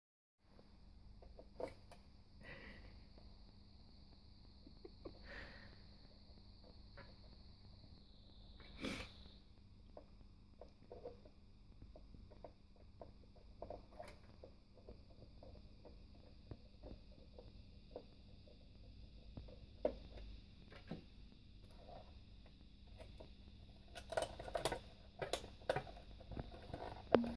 Albert 10vk apurina
Albert "Alpo" tuli 2pv sitten meille piikin alta ja on kyllä rohkea ja utelias kaveri. Oli heti miehen kaverina asentamassa antennijohtoa. Virtaa on kuin pienessä kylässä.